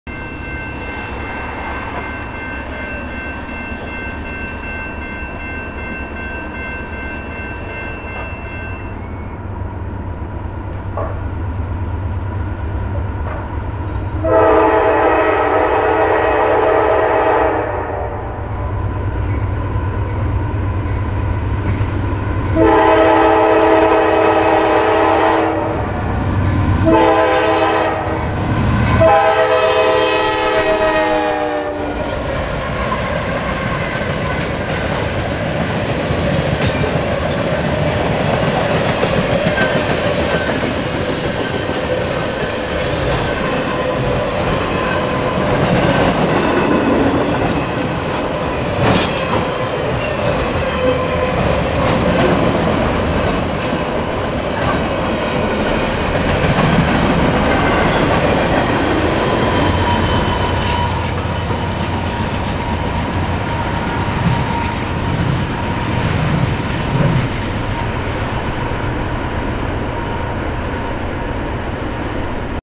NORFOLK SOUTHERN GE AC44C6M Southbound Manifest Mix Freight
A southbound Mix Freight Lead by Norfolk Southern GE AC44C6M #4624 Engine passing through at Milepost Rosemont.